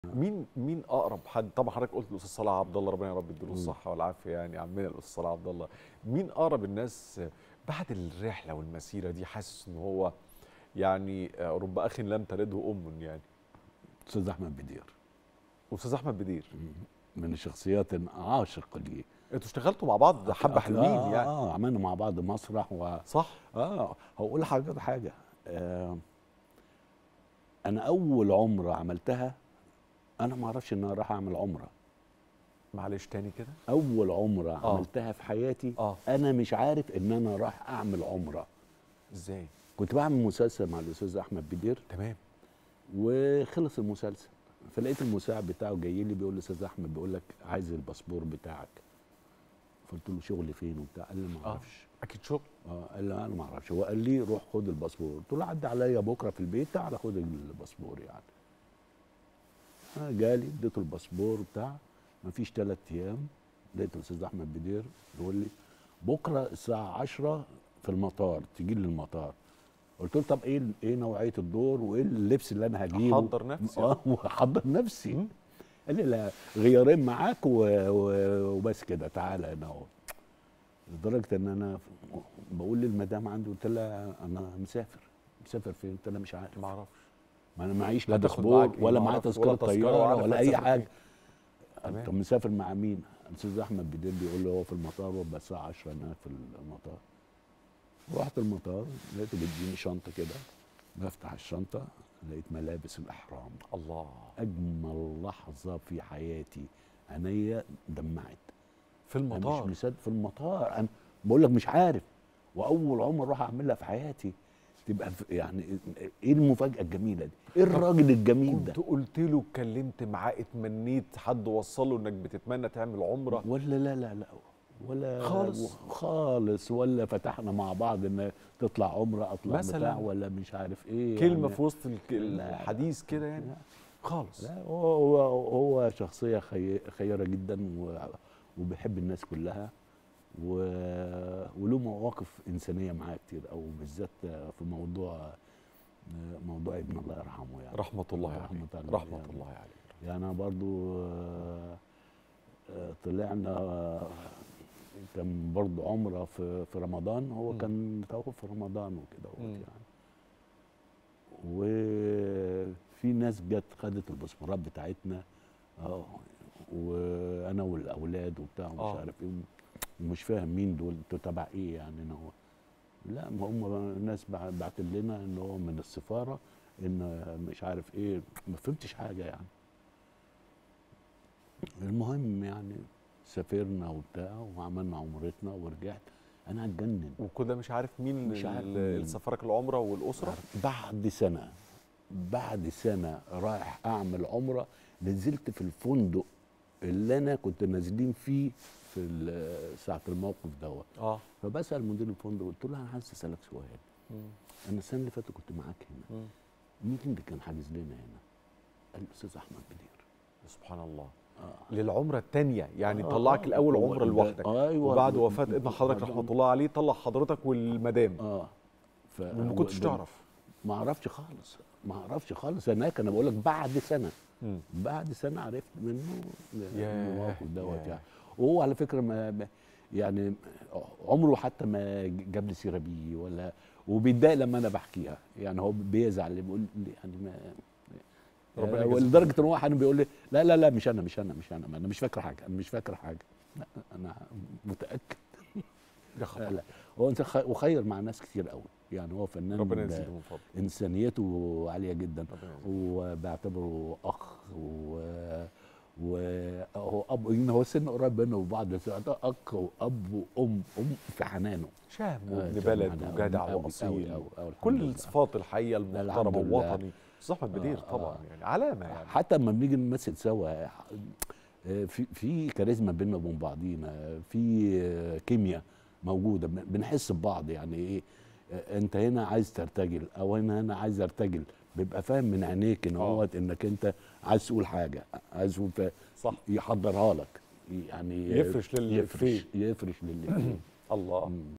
إيه الجمال ده.. صديق عمره فاجئه برحلة عمرة هدية.. محمد الصاوي يكشف عن أقرب الأصدقاء له
من مصر | إيه الجمال ده صديق عمره فاجئه برحلة عمرة هدية.. محمد الصاوي يكشف عن أقرب الأصدقاء له من الوسط الفني